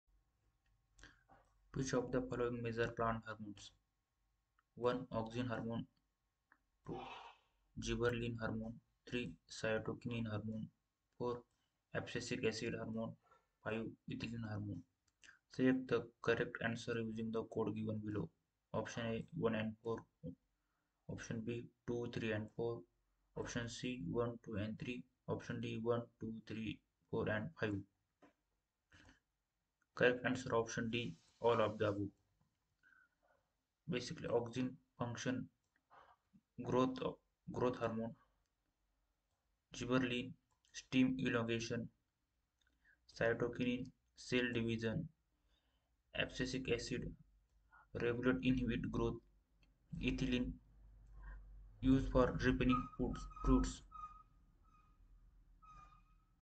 plant hormones | Auxin | Gibberellin | cytokinin | Abscisic acid | Ethylene
Q.1 Which of the following major plant hormones                                                                                                                                                                                                                                                                  1)  Auxin
2)  Gibberellin                                                                                                                                                                                                                                                                        3)  cytokinin                                                                                                                                                                                                                                                             4)  Abscisic acid
5)  Ethylene                                                                                                                                                                                                                   Select the correct answer using the codes given below:
(b) 2, 3 and 4
(d) 1, 2 , 3, 4 and 5
 Correct Answer: - (d) 1, 2 , 3, 4 and 5